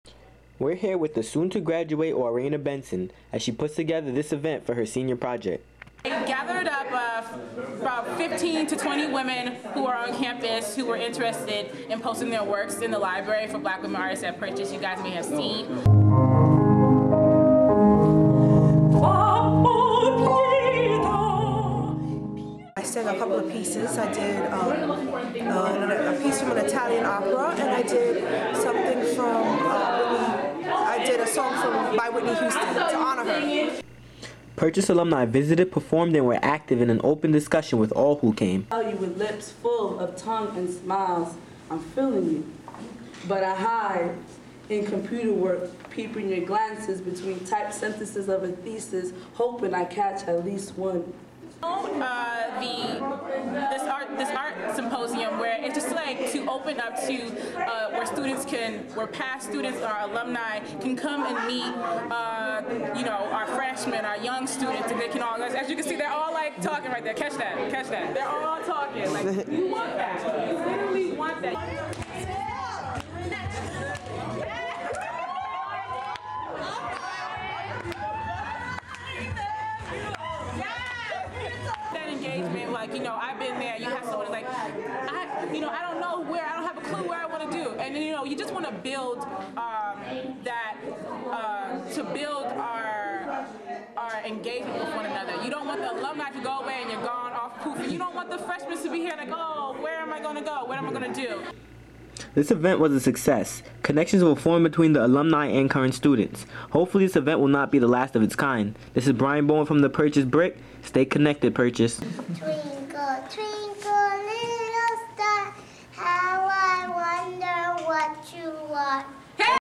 Black Women Artists at Purchase College
Senior Arts Management student, Woryeneh Benson, held her senior project: Black Women Artists at Purchase. The event bought alumni to back in an attempt to create a community between current students and graduates, but to also discuss the success and struggle of being a black woman artist. Brian Bowen reports.